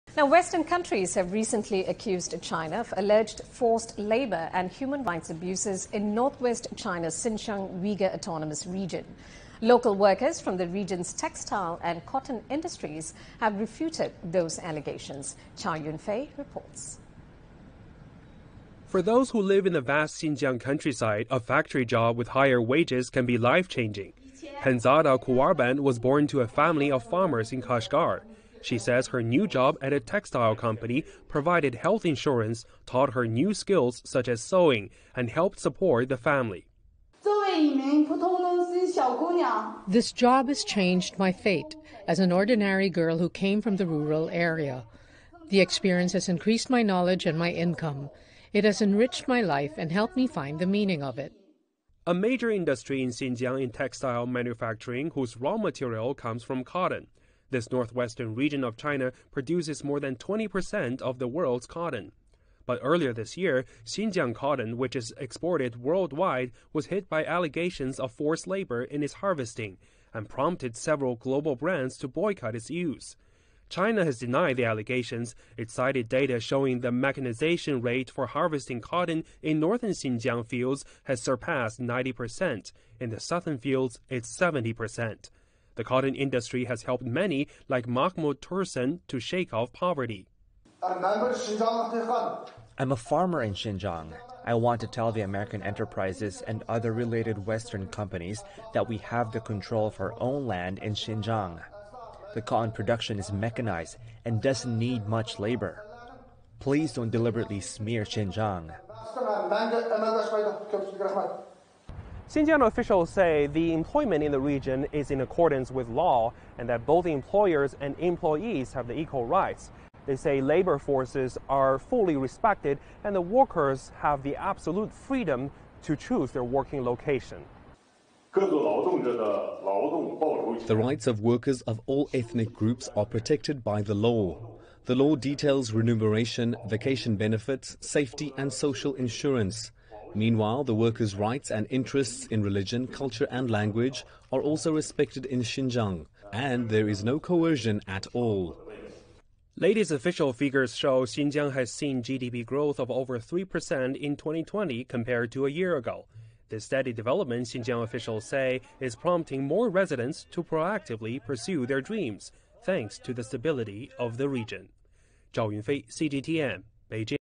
Xinjiang Issue Grassroots workers refute allegations of 'coercion'
Western countries have recently accused China of alleged forced labor and human rights abuses in northwest China's Xinjiang Uygur Autonomous Region. Local workers from the region's textile and cotton industries have refuted those allegations.